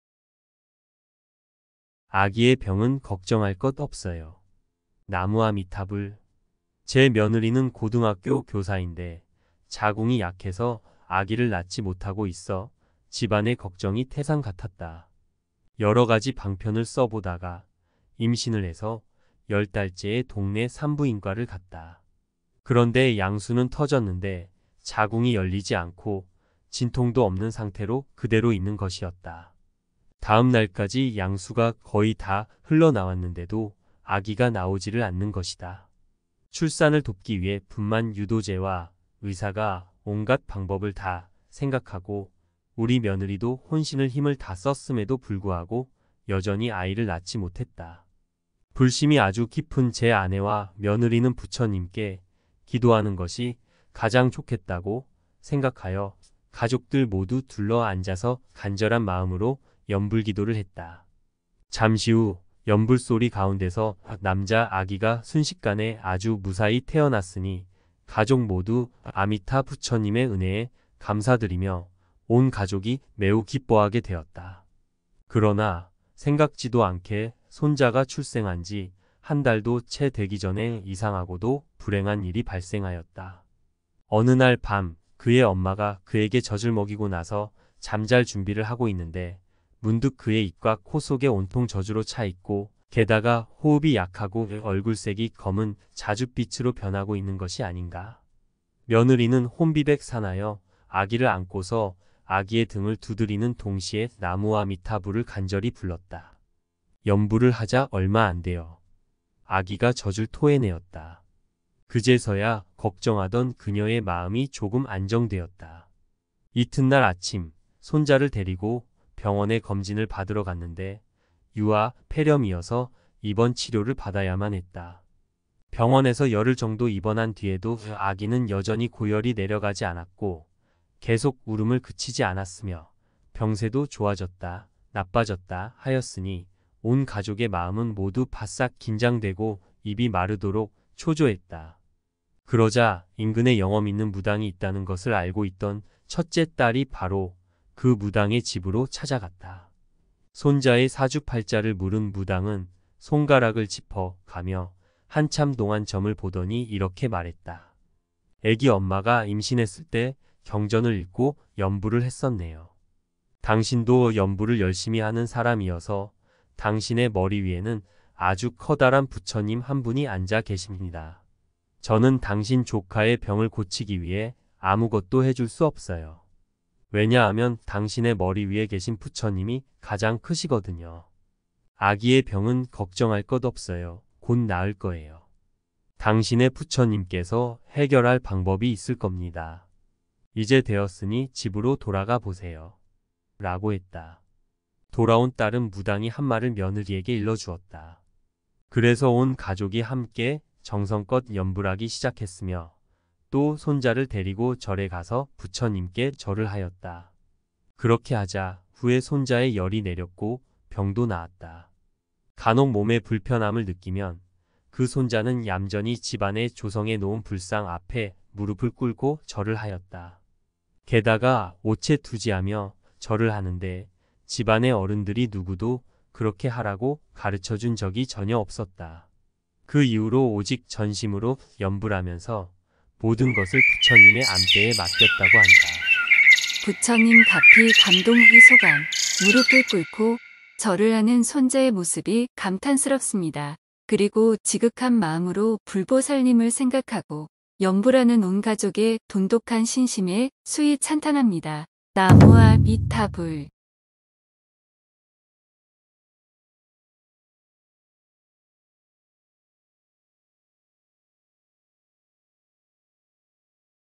여전히 아이를 낳지 못했다.불심이 아주 깊은 제 아내와 며느리는 부처님께 기도하는 것이 가장 좋겠다고 생각하여 가족들 모두 둘러 앉아서 간절한 마음으로#염불 기도를 했다.
아기의 병은 걱정할 것 없어요.나무아미타불~! 제 며느리는 고등학교 교사인데, 자궁이 약해서 아기를 낳지 못하고 있어 집안의 걱정이 태산 같았다.  여러가지 방편을 써보다가, 임신을 해서 10달째에 동네 산부인과를 갔다. 그런데 양수는 터졌는데 자궁이 열리지 않고 진통도 없는 상태로 그대로 있는 것이었다.  다음날까지 양수가 거의 다 흘러 나왔는데도 아기가 나오지를 않는 것이다.
 출산을 돕기 위해 분만 유도제와 의사가 온갖 방법을 다 생각하고 우리 며느리도 혼신을 힘을 다 썼음에도 불구하고 여전히 아이를 낳지 못했다. 불심이 아주 깊은 제 아내와 며느리는 부처님께 기도하는 것이 가장 좋겠다고 생각하여 가족들  모두 둘러 앉아서   간절한 마음으로 염불기도를 했다. 잠시 후 염불소리 가운데서 남자 아기가 순식간에 아주 무사히 태어났으니, 가족 모두 아미타부처님의  은혜에 감사드리며 온 가족이 매우 기뻐하게 되었다.  그러나 생각지도 않게 손자가 출생한지 한 달도 채 되기 전에 이상하고도 불행한 일이 발생하였다.  어느 날 밤, 그의 엄마가 그에게 젖을 먹이고 나서 잠잘 준비를 하고 있는데, 문득 그의 입과 코 속에 온통 젖으로 차 있고, 게다가 호흡이 약하고 얼굴색이 검은 자줏빛으로 변하고 있는 것이 아닌가?
며느리는 혼비백산하여 아기를 안고서 아기의 등을 두드리는 동시에  나무아미타불을 간절히 불렀다. 염불을 하지 얼마 안되어 아기가 젖을 토해내었다. 그제서야 걱정하던 그녀의 마음이 조금 안정되었다. 이튿날 아침, 손자를 데리고 병원에 검진을 받으러 갔는데, 유아 폐렴이어서 입원치료를 받아야만 했다. 병원에서 열흘 정도 입원한 뒤에도 아기는 여전히 고열이 내려가지 않았고, 계속 울음을 그치지 않았으며, 병세도 좋아졌다 나빠졌다 하였으니, 온 가족의 마음은 모두 바싹 긴장되고 입이 마르도록 초조했다. 그러자 인근에 영험 있는 무당이 있다는 것을 알고 있던 첫째 딸이 바로 그 무당의 집으로 찾아갔다. 손자의 사주팔자를 물은 무당은 손가락을 짚어 가며 한참동안 점을 보더니 이렇게 말했다. “애기 엄마가 임신했을 때 경전을 읽고 염불을 했었네요. 당신도 염불을 열심히 하는 사람이어서 당신의 머리위에는 아주 커다란 부처님 한 분이 앉아 계십니다!  저는 당신 조카의 병을 고치기 위해 아무 것도 해 줄 수 없어요. 왜냐하면 당신의 머리 위에 계신 부처님이  가장 크시거든요. 아기의 병은 걱정할 것 없어요, 곧 나을 거에요.  당신의 부처님께서 해결할 방법이 있을 겁니다. 이제 되었으니 집으로 돌아가 보세요!”라고 했다.  돌아온 딸은 무당이 한 말을 며느리에게 일러주었다. 그래서 온 가족이 함께 정성껏 염불하기 시작했으며, 또 손자를 데리고 절에 가서 부처님께 절을 하였다. 그렇게 하자, 후에 손자의 열이 내렸고 병도 나았다.  간혹 몸에 불편함을 느끼면 그 손자는 얌전히 집안에 조성해 놓은 불상 앞에 무릎을 꿇고 절을 하였다.  게다가 오체투지하며 절을 하는데 집안의 어른들이 누구도 그렇게 하라고 가르쳐준 적이 전혀 없었다.
그 이후로 오직 전심으로 염불하면서 모든 것을 부처님의 안배에 맡겼다고 한다. 부처님 가피 감동 후기 소감 : 무릎을 꿇고 절을 하는 손자의 모습이 감탄스럽습니다. 그리고 지극한 마음으로 불보살님을 생각하고 염불하는 온 가족의 돈독한 신심에 수희 찬탄합니다. " 성불하세요."나무아미타불